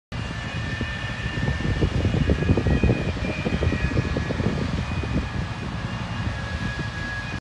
Devastador incendio sobre una fábrica de plásticos del Parque Industrial
En horas de la tarde se produjo un impresionante y devastador incendio sobre la fábrica “Plásticos de la Isla Grande S. A.” ubicada sobre calle Ortiz, entre Almirante Brown y Padre Forgacs.
Numerosas dotaciones de bomberos, personal policial y defensa civil se encuentran trabajando en el lugar, donde las llamas tomaron la totalidad de los galpones. La nube de humo negro se logra observar desde gran parte de la ciudad.